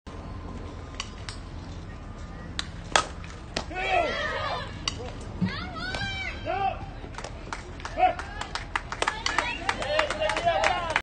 Riley Williams RHP 2022 So Cal Athletics Limo PITCHING vs Beverly Bandits Colorado IDT July 2019 3
Fielding the Position